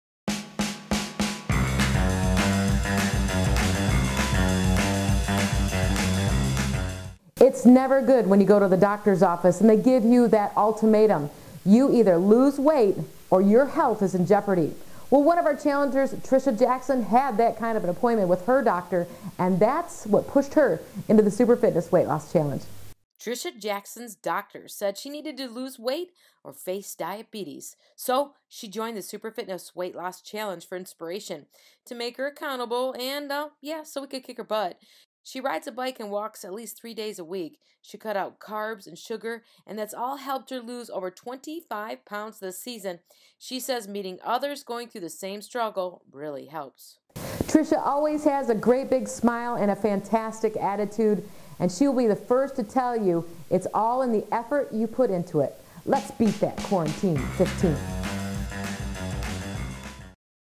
Challenger Spotlight: Tricia Jackson | SFWLC
Kelly Heidbreder shines the spotlight on one of the Super Fitness Weight Loss Challengers.